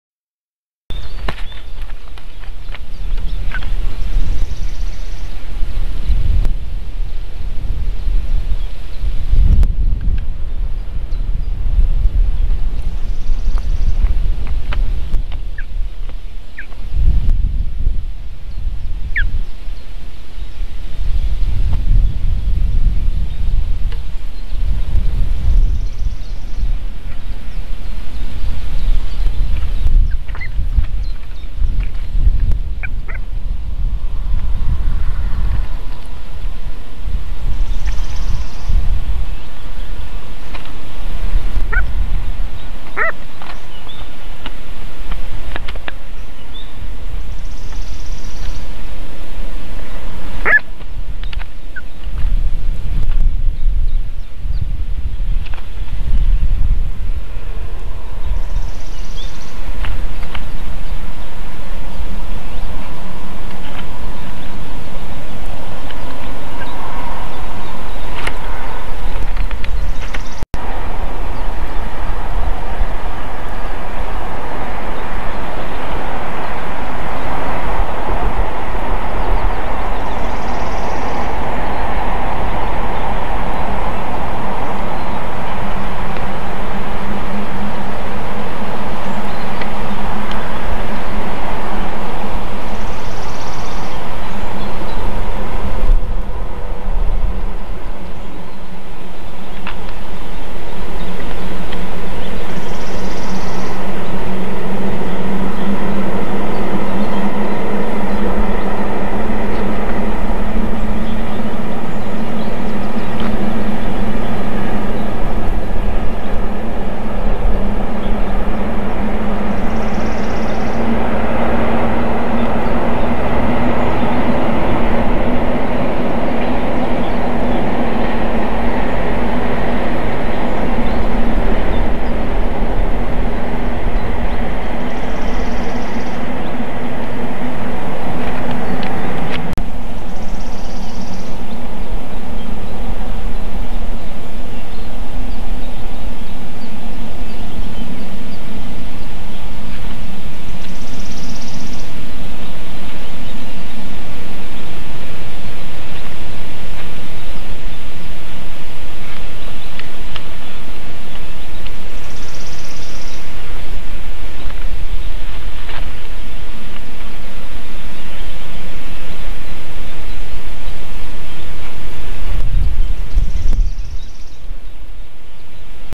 M1 in front of the cam (kalakotkas | Marko + Miina)
Nest is located in South Estonia, Võrumaa.
A fish farm, as well as a river, is not far – so good hunting conditions for ospreys.

Look for the current address at the channel of Eagle Club Estonia / Kotkaklubi ("Kalakotkas::Pandion haliaetus"):